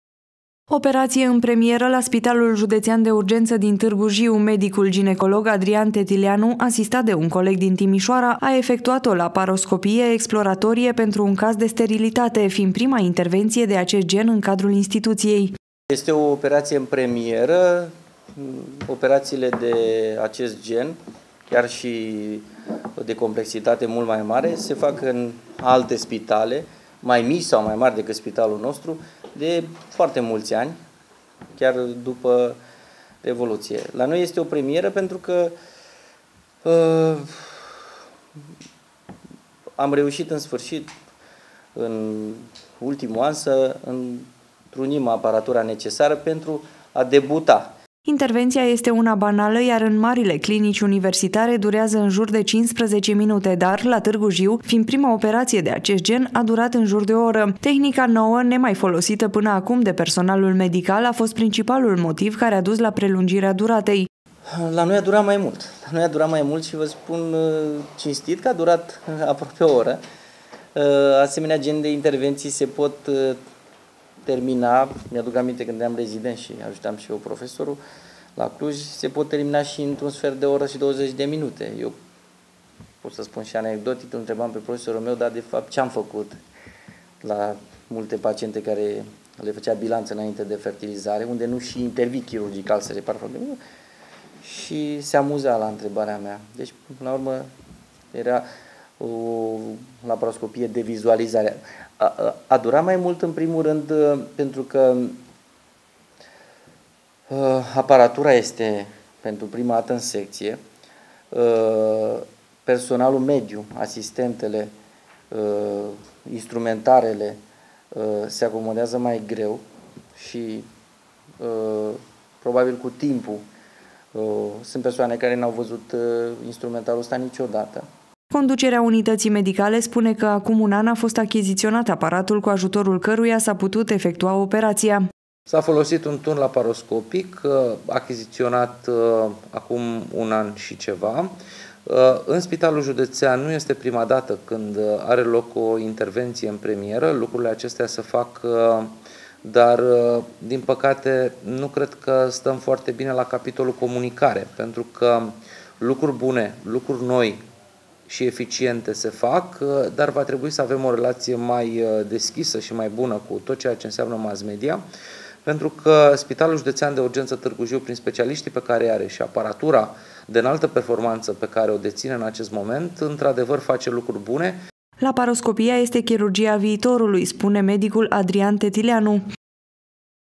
OPERAȚIE ÎN PREMIERĂ LA TÂRGU JIU
A fost efectuată o laparoscopie exploratorie pentru un caz de sterilitate